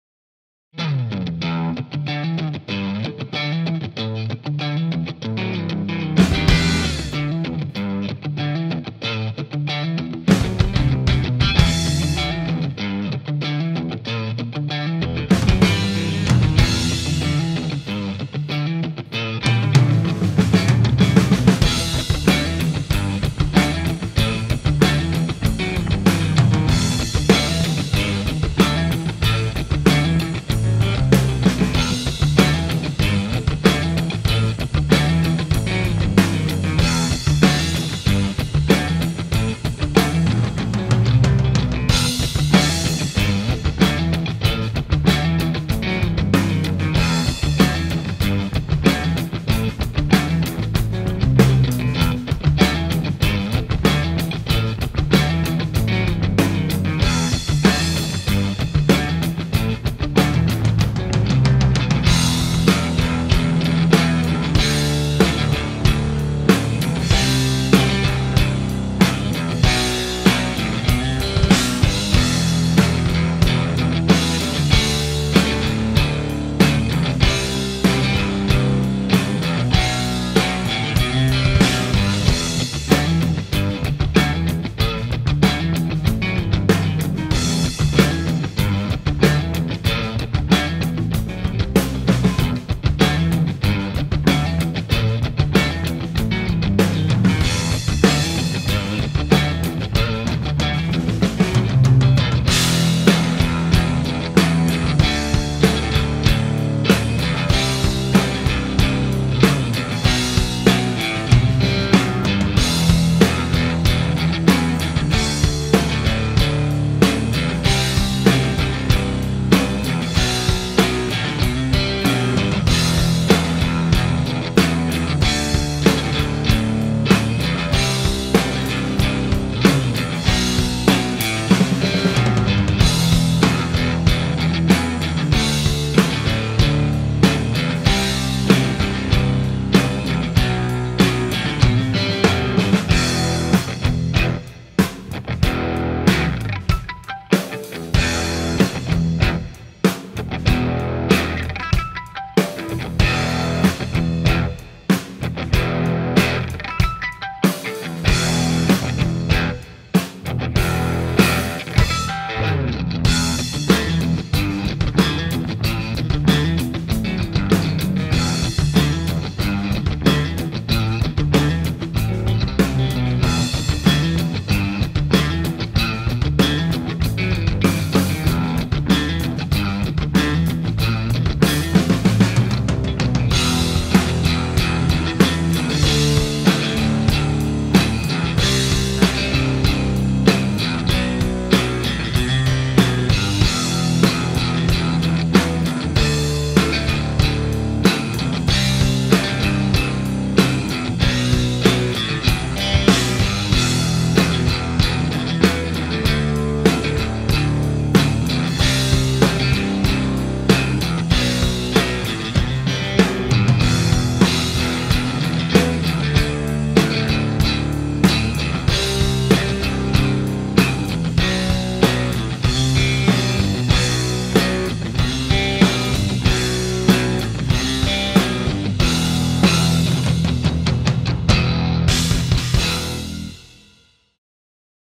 Classic Funky Rock in "E" - Guitar Backing Track
This is a really fun backing track to jam along to in "E". Thanks for all of the support!